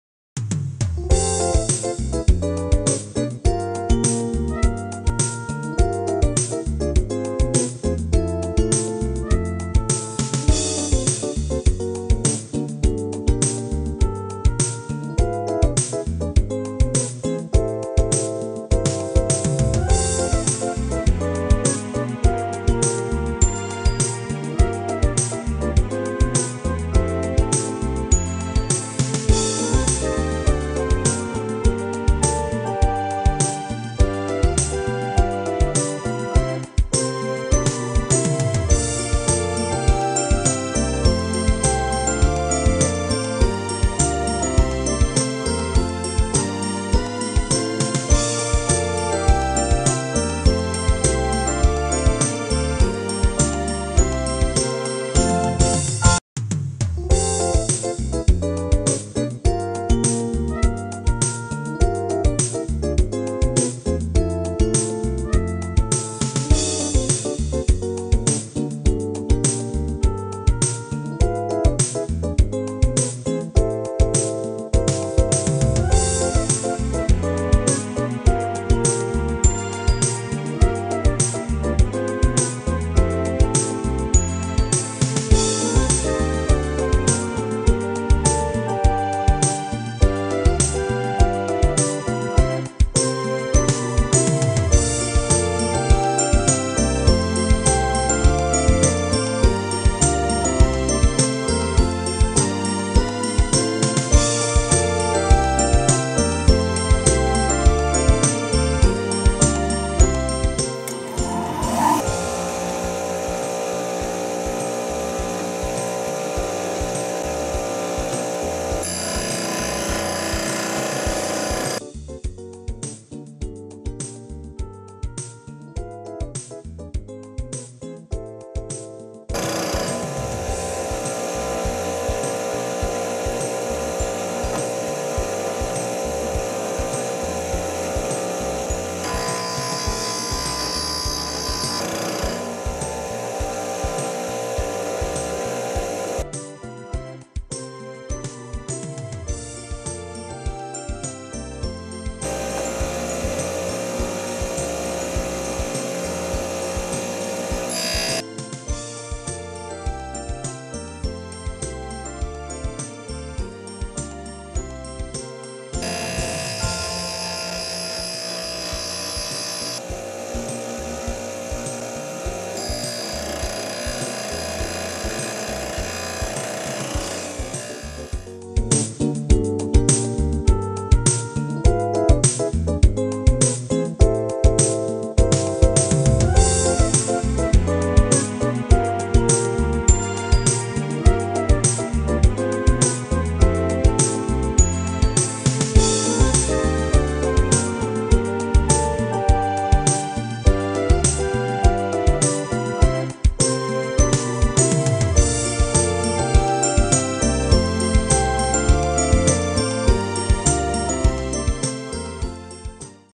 ドリ研X26
大径ドリル研磨機の決定版！
従来のドリル研磨機では不可能だったΦ26までのドリルに対応し、シンニング（チゼル幅）の調整も実現しました！
詳しくは以下製品ページをご覧下さい。